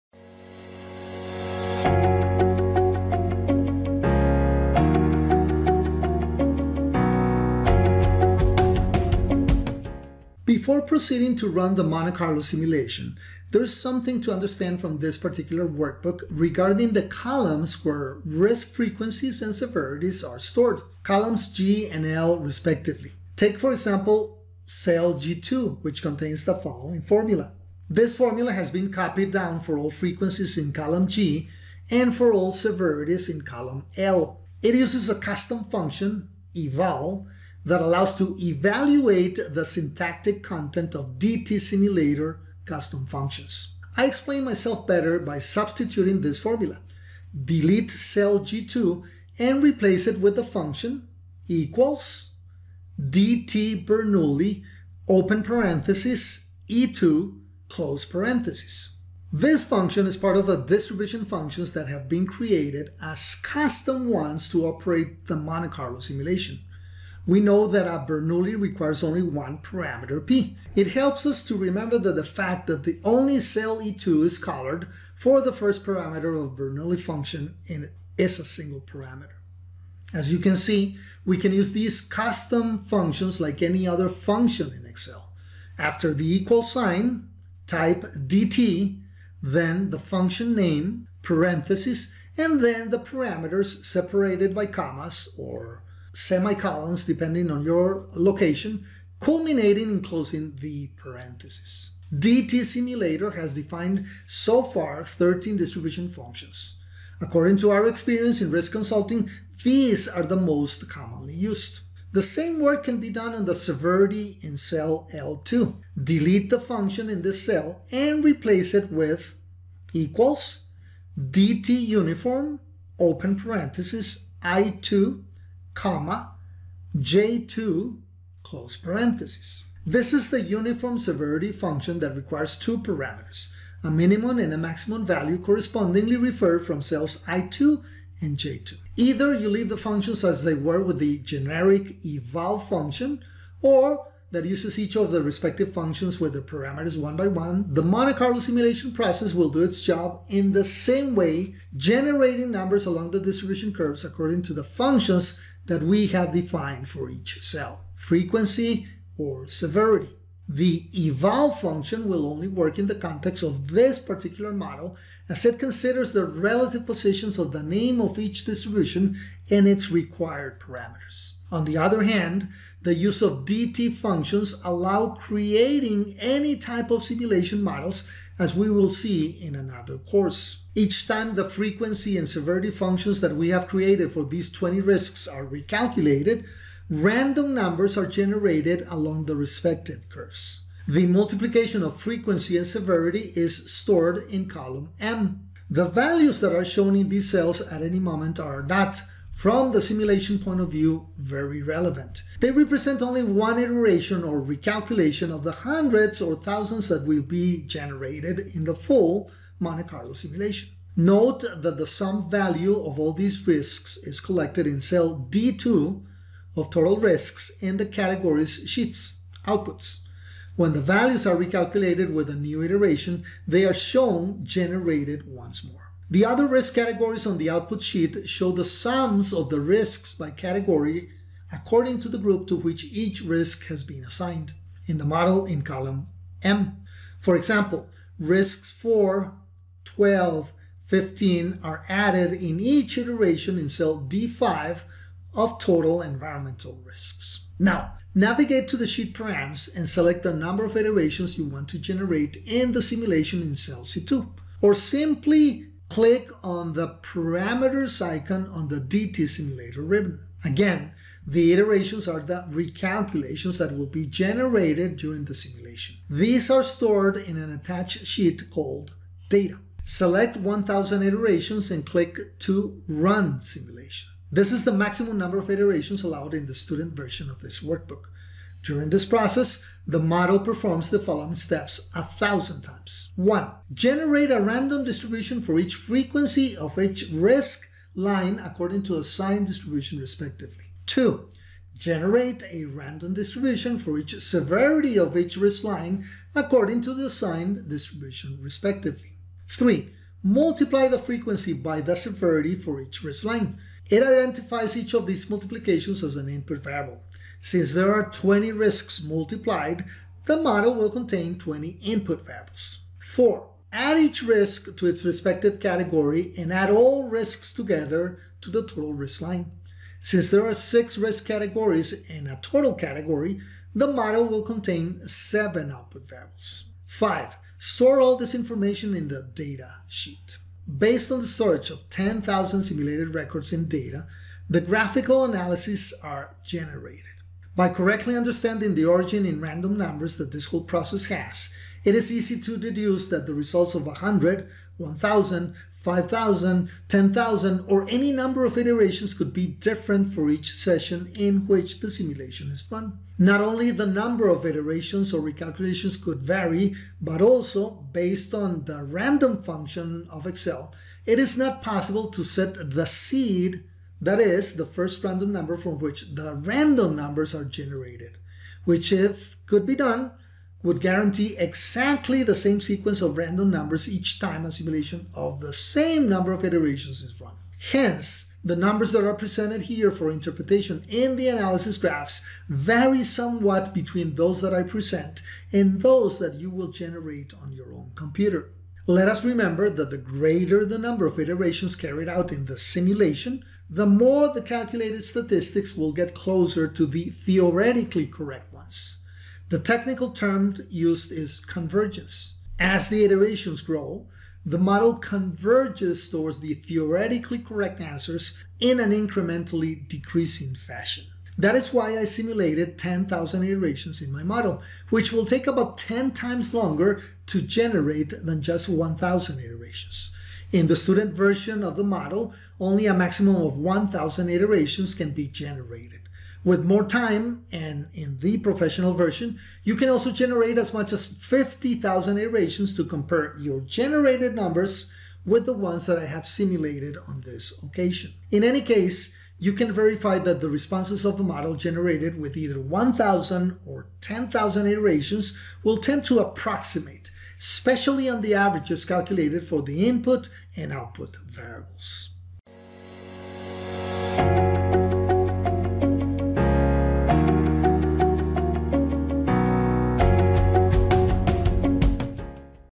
Quantitative Project Risk Analysis in Excel Lesson 08
LESSON 8: RUNNING THE MONTE CARLO SIMULATION
Before proceeding to run the Monte Carlo simulation, there is something to understand from this particular workbook regarding the columns where hazard frequencies and severities are stored: columns G and L respectively.

This formula has been copied for all frequencies in column G and for all severities in column L. It uses a custom function that allows to evaluate the syntactic content of DTSimulator custom functions.
I explain myself better by substituting this formula. Delete cell G2 and replace it with the function
  = DTBernoulli (E2)
This function is part of the distribution functions that have been created as custom functions to operate the Monte Carlo simulation. We know that a Bernoulli requires only one parameter p. It helps us to remember that the fact that only cell E2 is colored for the first parameter of a Benoulli function is a single parameter.
As you can see, we can use these custom functions like any other function in Excel: After the equal sign (“=”), type “DT”, then the function name, parentheses, and then the parameters separated by commas (or period and commas depending on your locale), culminating in closing the parentheses.
DTSimulator has defined thirteen distribution functions so far. According to our experience in risk consulting, these are the most commonly used: